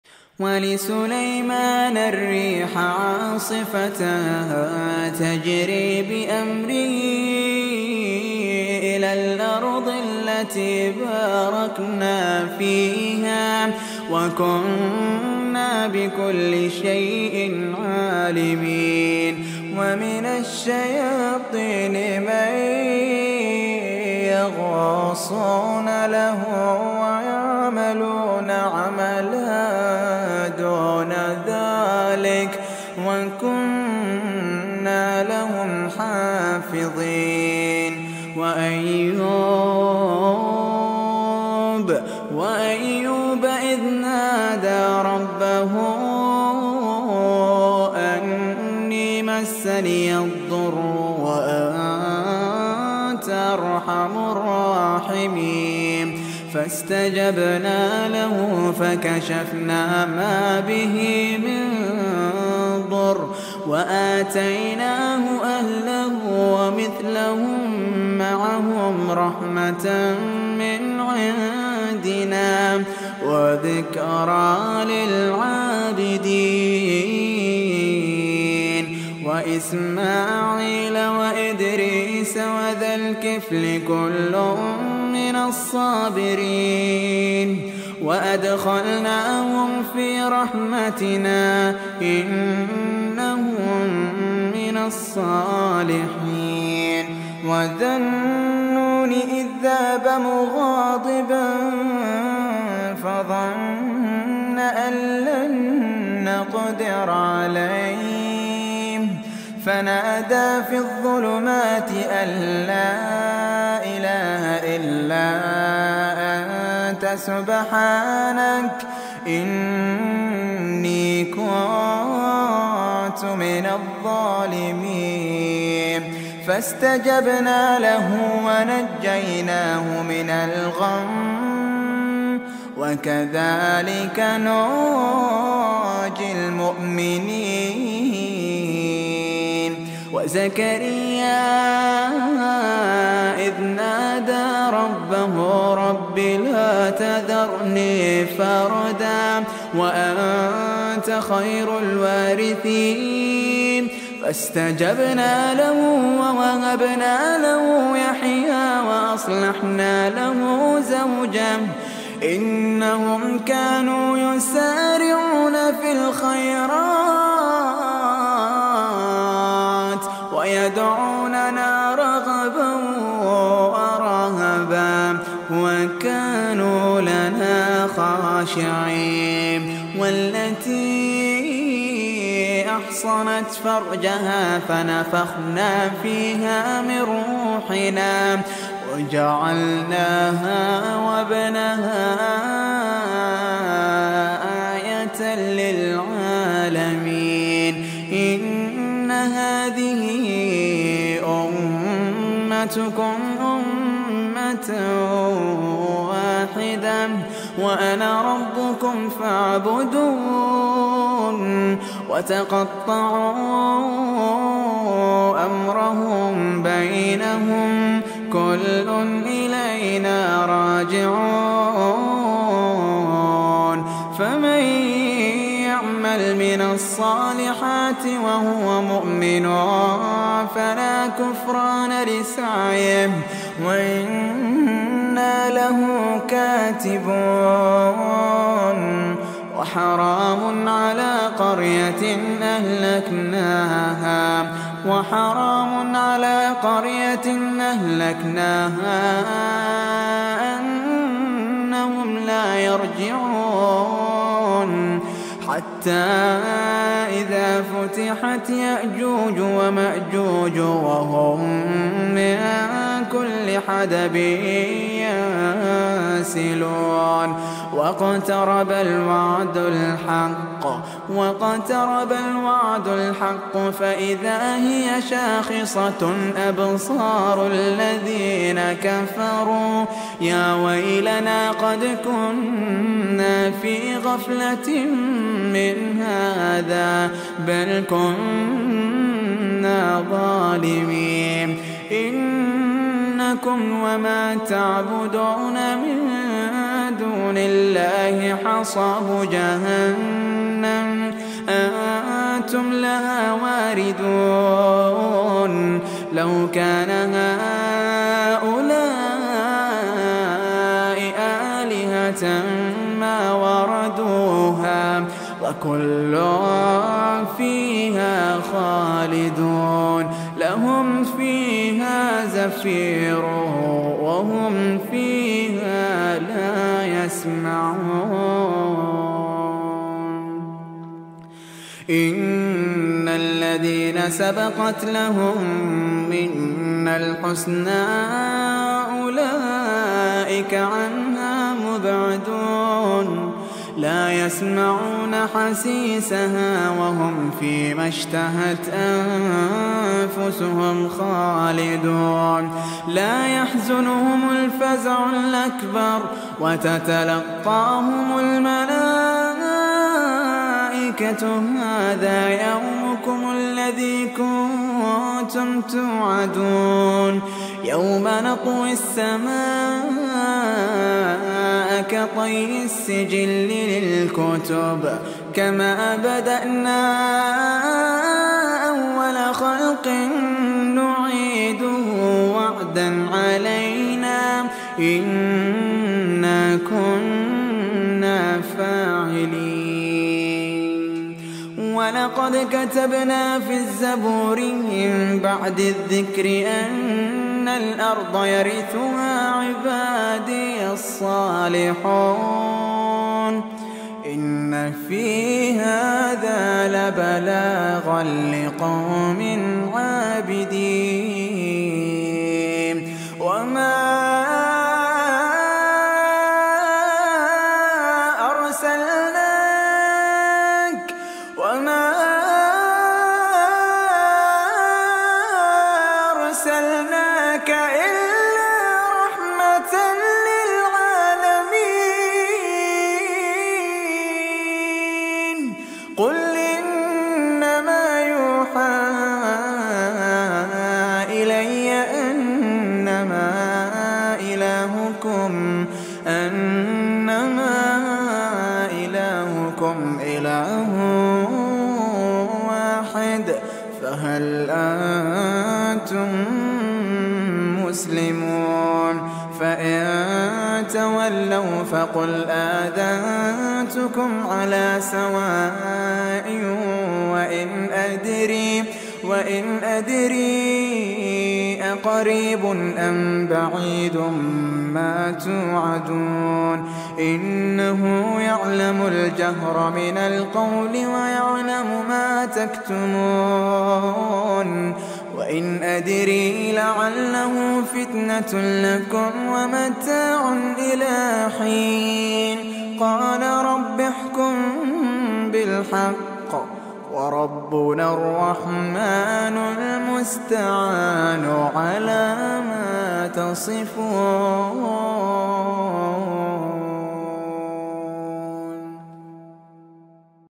تلاوة عذبة يرق لها القلب من سورة الانبياء😍🌸 || شريف مصطفى❤️
القناة الرسمية للقارئ شريف مصطفى

القرآن الكريم:
 آيات من سورة الأنبياء
تلاوة عذبة يرق لها القلب قمة في الجمال
تواصلوا معنا | القارئ شريف مصطفى